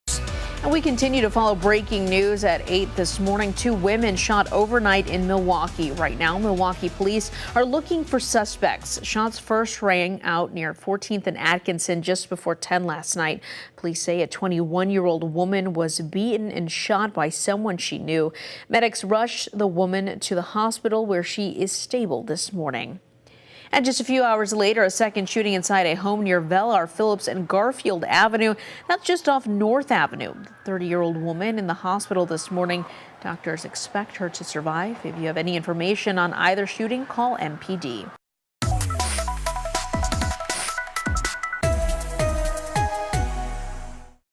Two women shot over weekend expected to survive
Two women were shot in Milwaukee on Saturday night and Sunday morning.